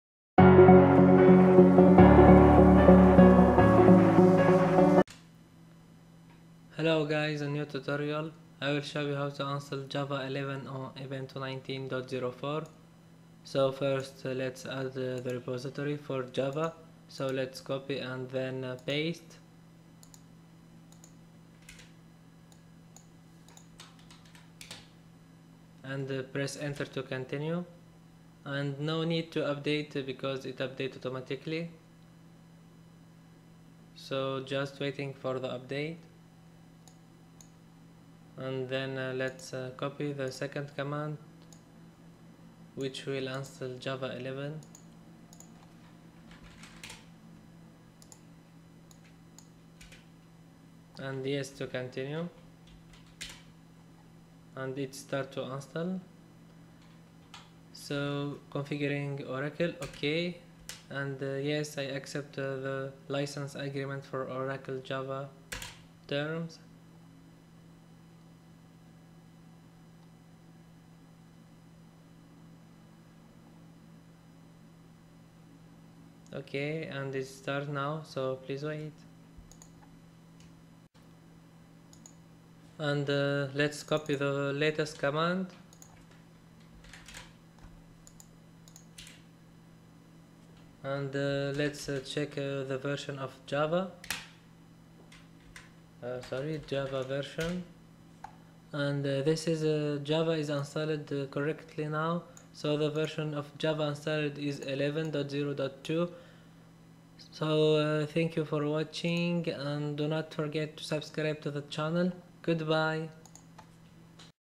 How To Install Java 11 on Ubuntu 19.04
On this video , I will show you How To Install java 11 on Ubuntu 19.04 Step By Step.

►Commands

sudo add-apt-repository ppa:linuxuprising/java

sudo apt-get install oracle-java11-installer

sudo apt-get install oracle-java11-set-default